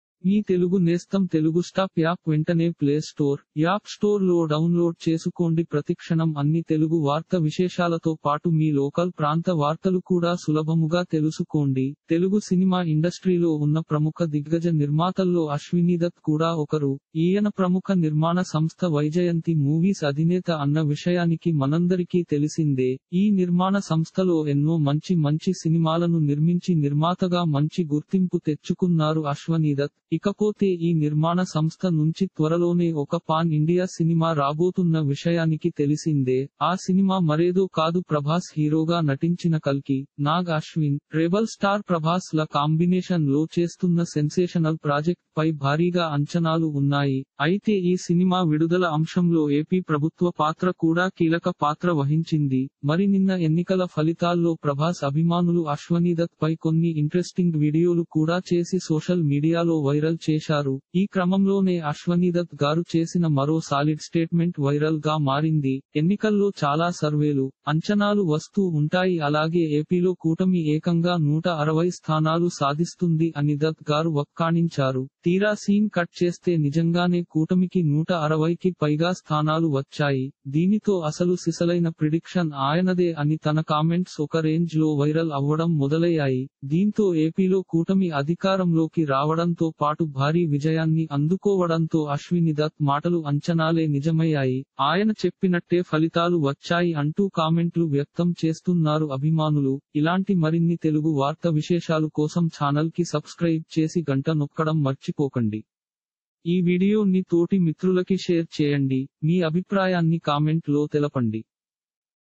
Viral-kalki-2898-ad-producer-ashwini-dutt-predicted-exact-ap-results - ఆ నిర్మాత అంచనాలే నిజం.. ఏప
తెలుగు సినిమా ఇండస్ట్రీలో ఉన్న ప్రము - Viral-kalki-2898-ad-producer-ashwini-dutt-predicted-exact-ap-results $telugu-title:ఆ నిర్మాత అంచనాలే నిజం.. ఏపీలో ఆయన చెప్పినట్టే ఫలితాలు వచ్చాయిగా!$ *pid:2915812* https:/ - Telugu Movies TeluguMovie Tollywood TollywoodNews TeluguMovieNews Telugu TeluguStop | Movie